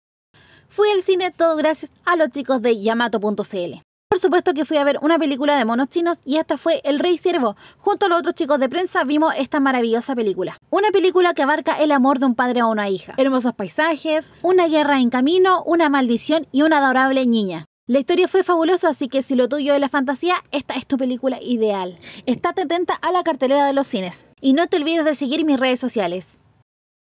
Pre estreno "El rey ciervo"
Tuve la suerte de asistir al estreno de prensa de esta maravillosa película, ustedes ya la vieron ?